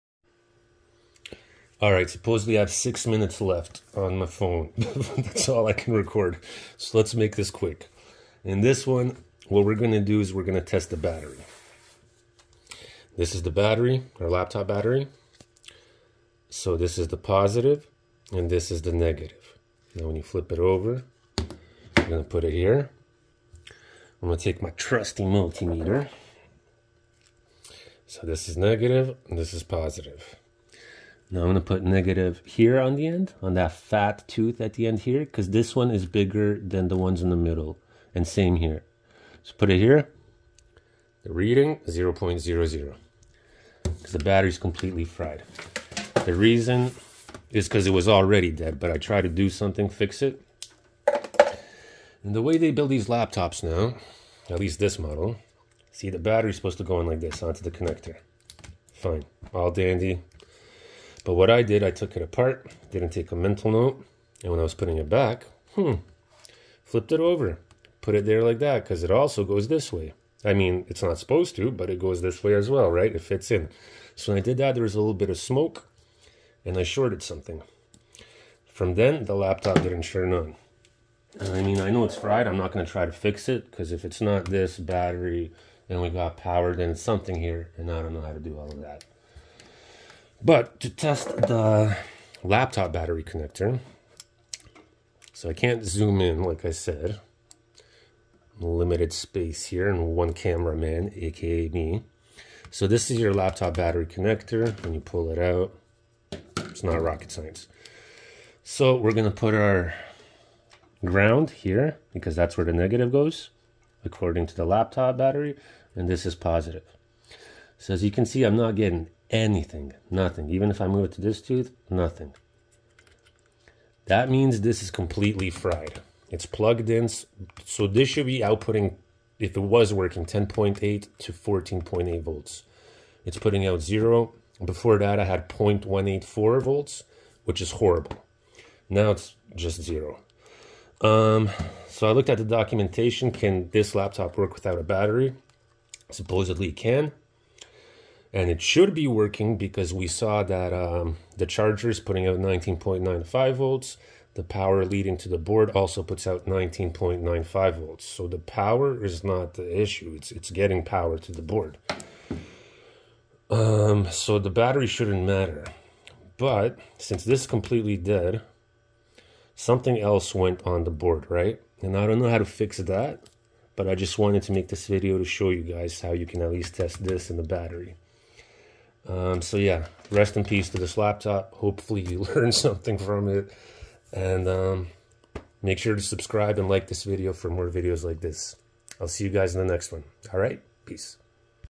How To Check Laptop Battery With Multimeter
In this video we be looking at how to check laptop battery with multimeter. we will also test the battery connector.